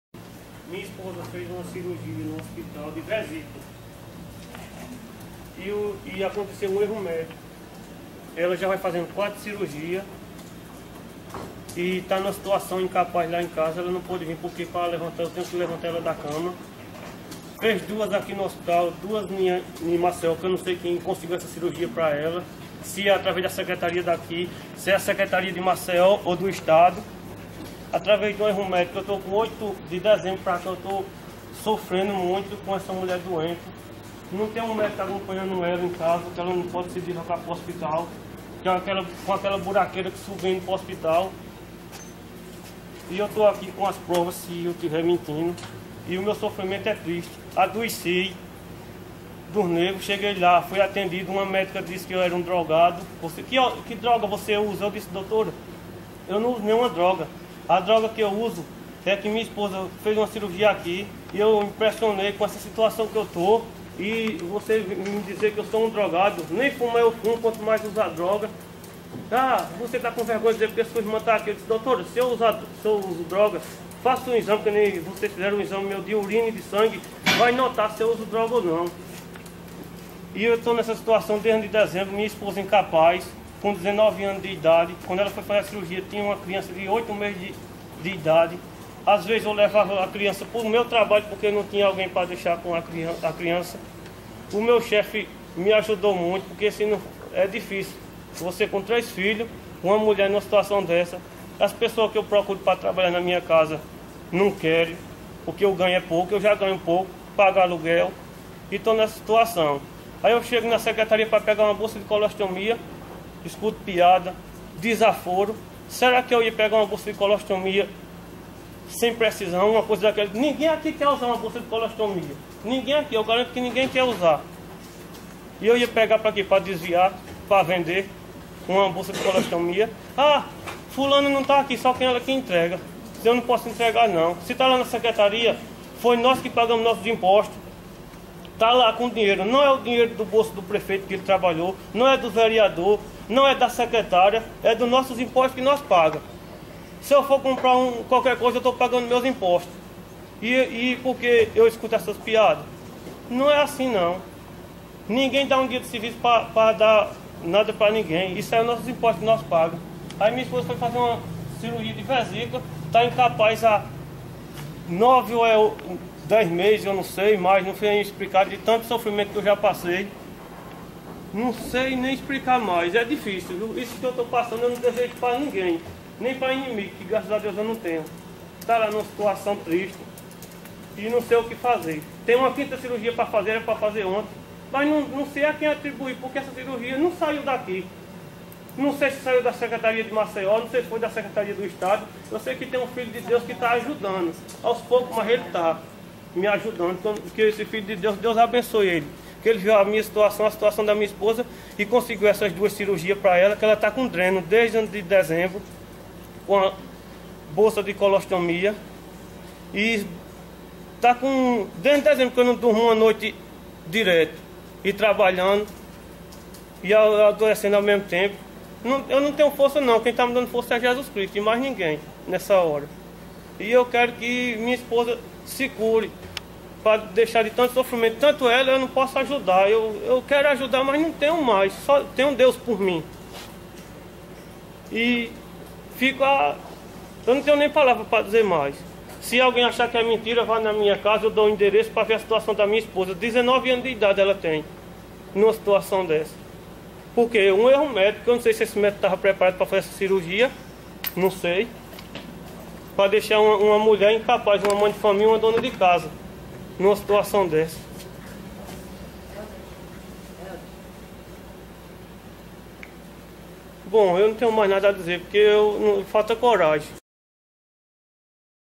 Mais um erro médico em Santana deixa mulher em estado grave 2014
Usuário do SUS. Josival, denuncia ao Grupo de Trabalho (GT) de Defesa da Saúde Pública do Ministério Público do Estado de Alagoas (MPE/AL) erro médico no Hospital regional em Santana do Ipanema. esposa com 19 anos,está sofrendo há 8 meses depois de realizar uma cirurgia de vesícula. Vem fazendo 4 novas cirurgias para reparar danos no seu corpo. A denúncia foi durante a  audiência pública em prol de garantir uma melhor prestação de serviço de saúde em Santana do Ipanema, Alagoas, no dia 15 de agosto de 2014, no auditório do Fórum. A coordenadora do GT Dra. Micheline Tenório disse que o objetivo é obter um diagnóstico da prestação dos serviços de saúde pública existentes no município. Presentes o Promotor Dr. Hamilton Carneiro e o juiz de direito Dr. Durval Mendonça da comarca de Santana do Ipanema, conselheiros municipais de saúde, prefeito, funcionários da secretaria de saúde e do hospital regional, usuário do SUS e vereadores.